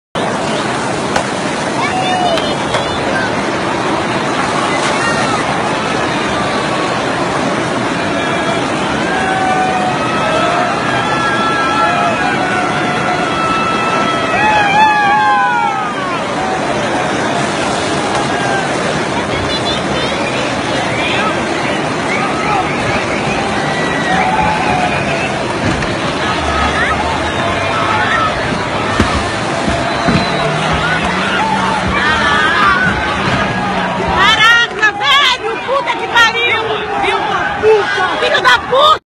MOTORISTA ATROPELA SKATISTAS EM SP
ÓDIO ATROPELA SKATISTAS